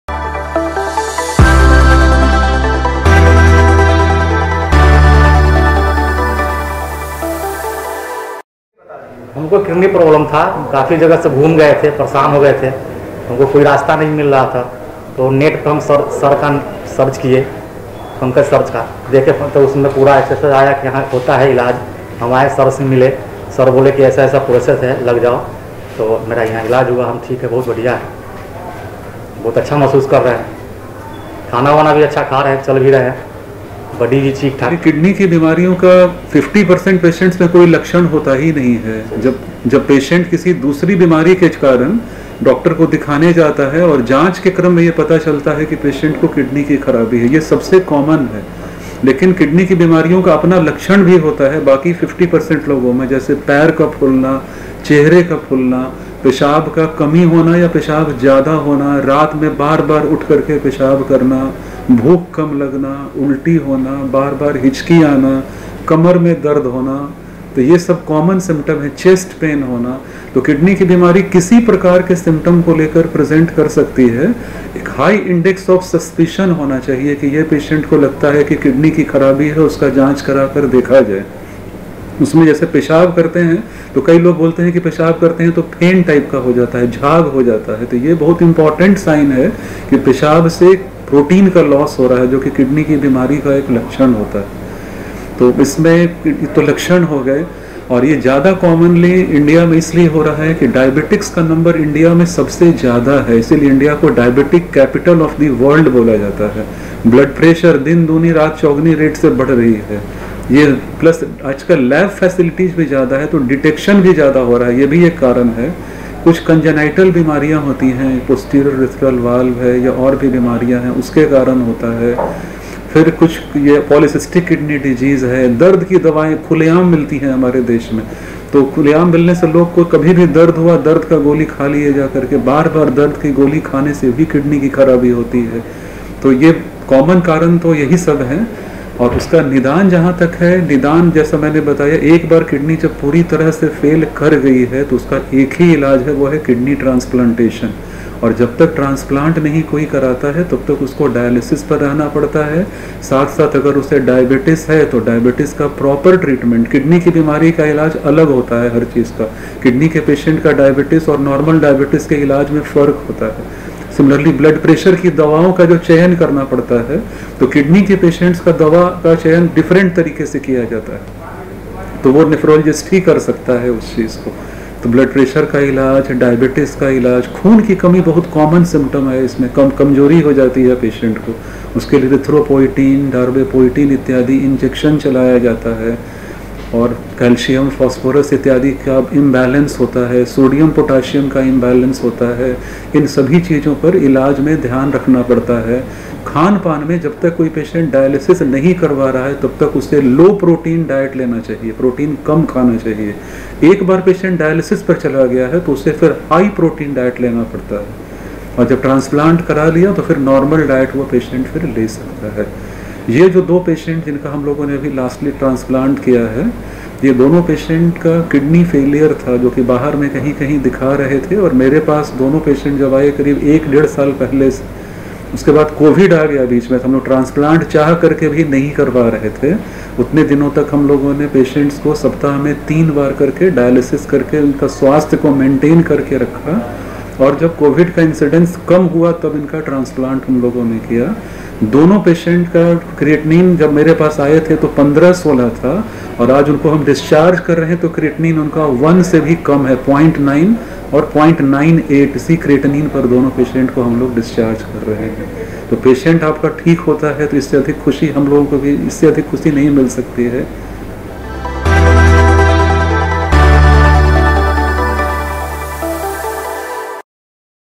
किडनी खराब होने के संकेत और उपाय /पटना के Ruban Memorial Hospital में सफल किडनी ट्रांसप्लांट
Dr Pankaj Hans
 HOD, Nephrology
Ruban Memorial Hospital